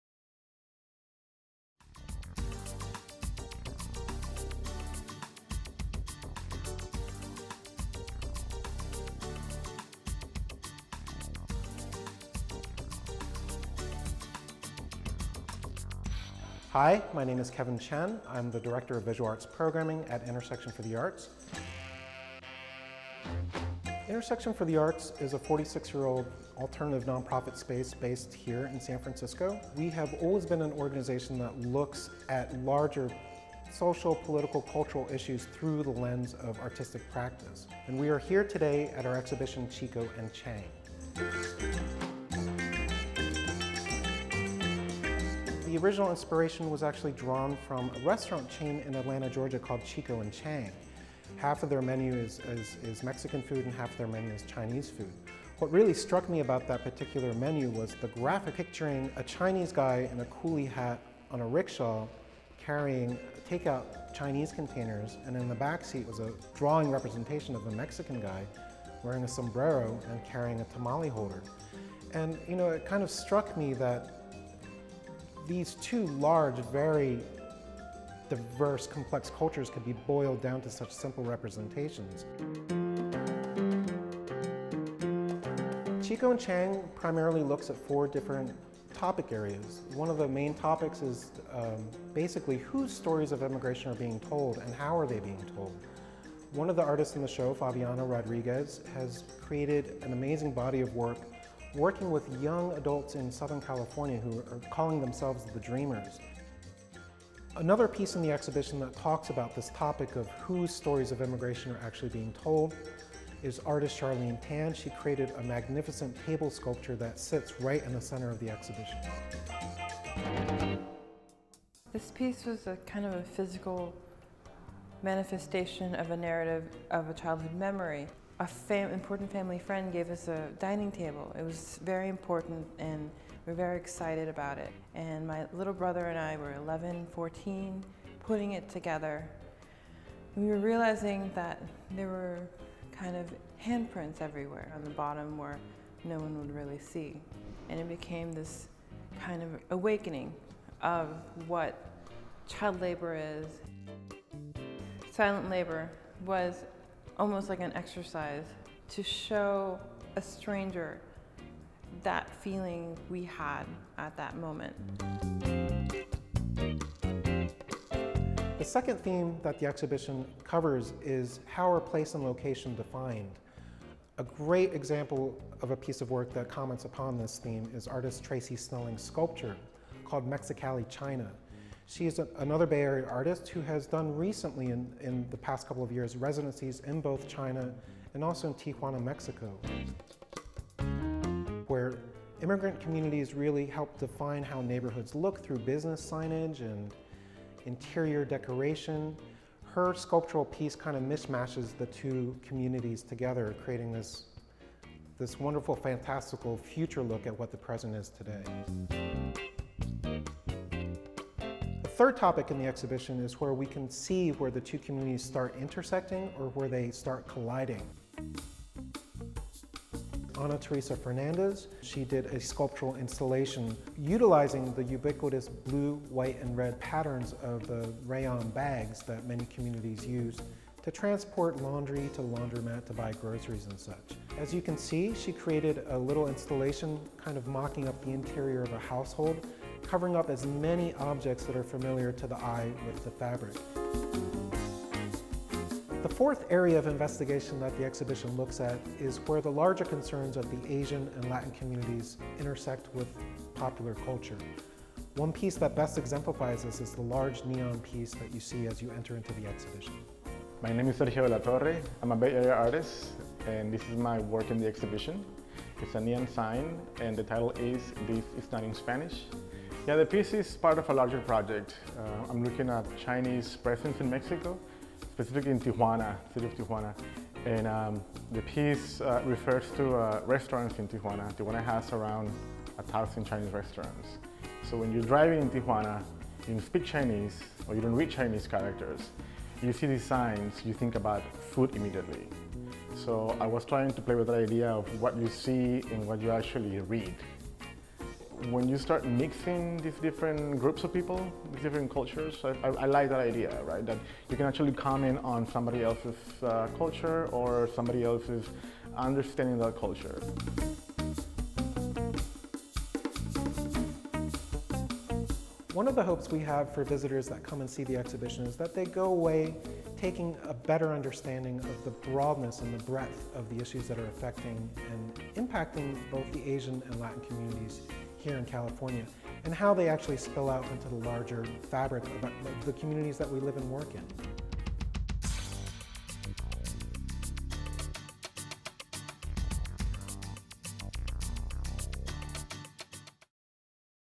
Intersection for the Arts: Chico & Chang
Intersection for the Arts presents Chico & Chang, a group exhibition that looks at the impact of Latino and Asian cultures on California's visual landscape through new sculpture, video, and painting. Director of Visual Arts Programming, Kevin Chen, introduces the exhibit's four topic areas and two of the artists, Charlene Tan and Sergio De La Torre.